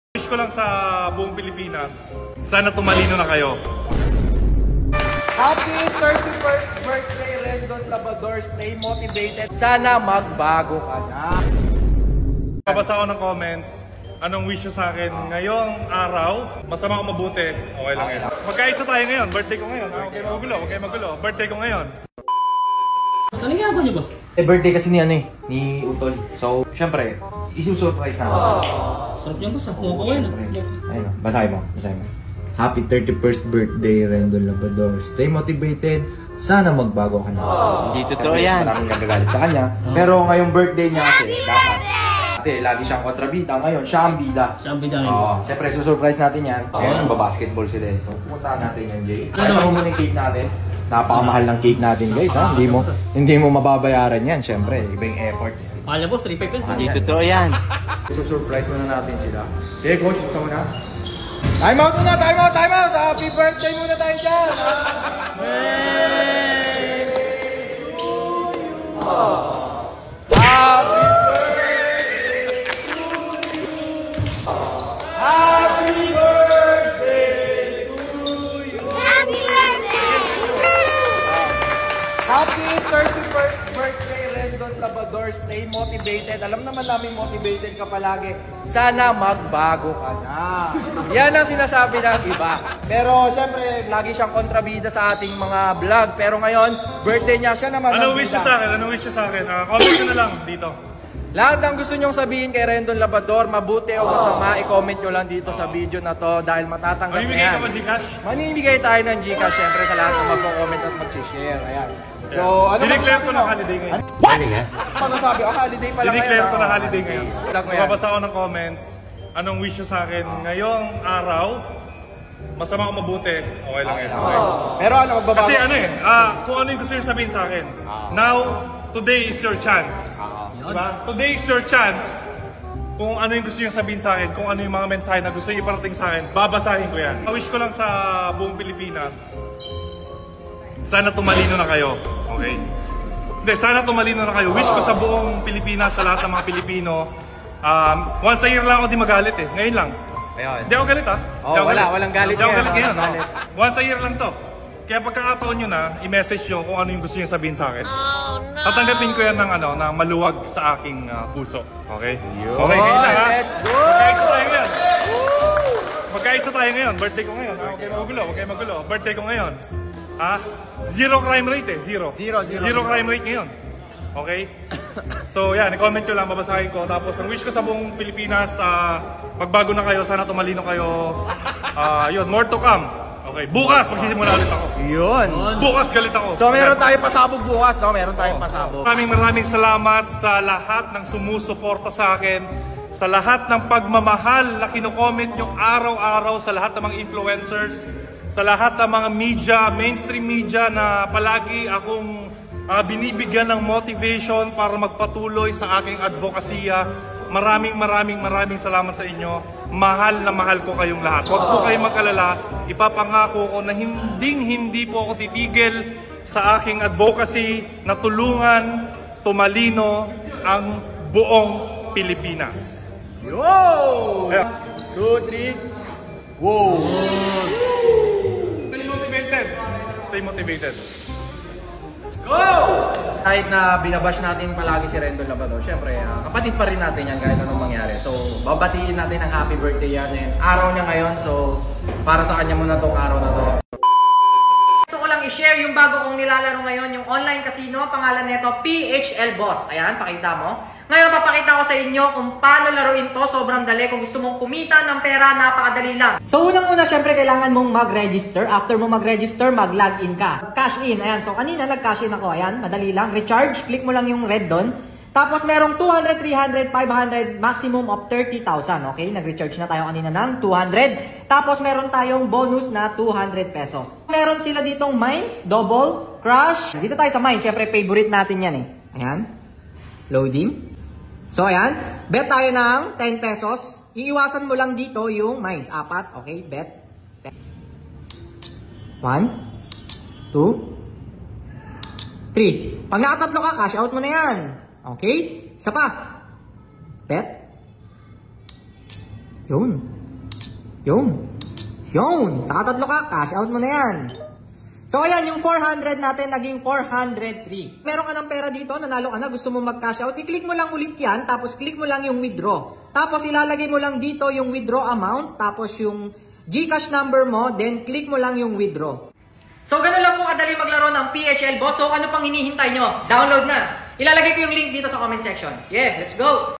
📣 RENDON LABADOR B-DAY SURPRISE 😅
📣 Wish nya tumalino ang mga pilipino sa B-Day nya! Kayo anong wish nyo para kay Rendon Labador? 😅